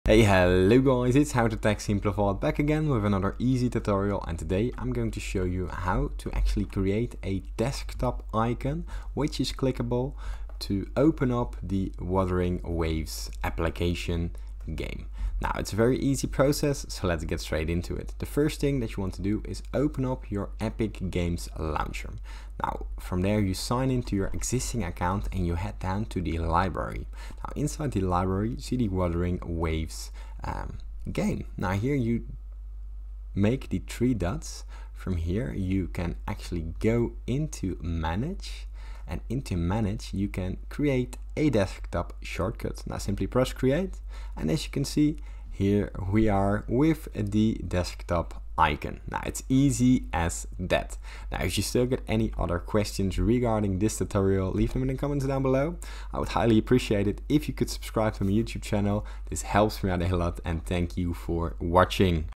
How To Create Desktop Shortcut for Wuthering Waves
Learn how to create a convenient desktop shortcut for Wuthering Waves, ensuring quick access to your favorite game. Follow our simple instructions to set up a desktop icon for Wuthering Waves, allowing you to launch the game with just a single click. Streamline your gaming experience and save time with this easy-to-follow tutorial. Elevate your Wuthering Waves journey by having your desktop icon ready whenever you're ready to embark on your adventure.